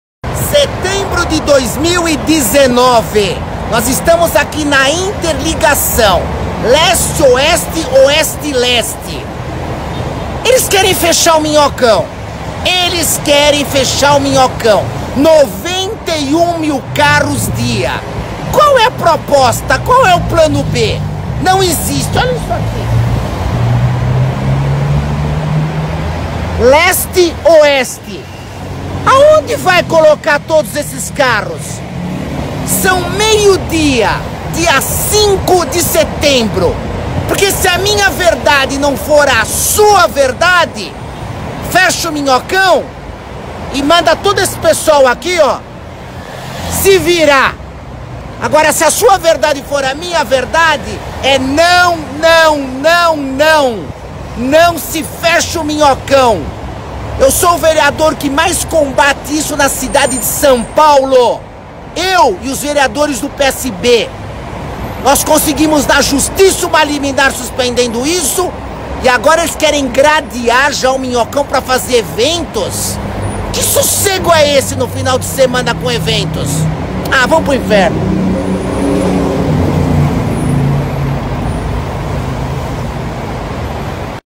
Fechamento do Minhocão • Vereador Camilo Cristófaro
Camilo Cristófaro fala sobre o fechamento do Minhocão, um dos m ais importantes corredores de interligação entre bairros da cidade.

📣 Camilo Cristófaro 📣
CONHEÇA O TRABALHO COMPLETO DESTE VEREADOR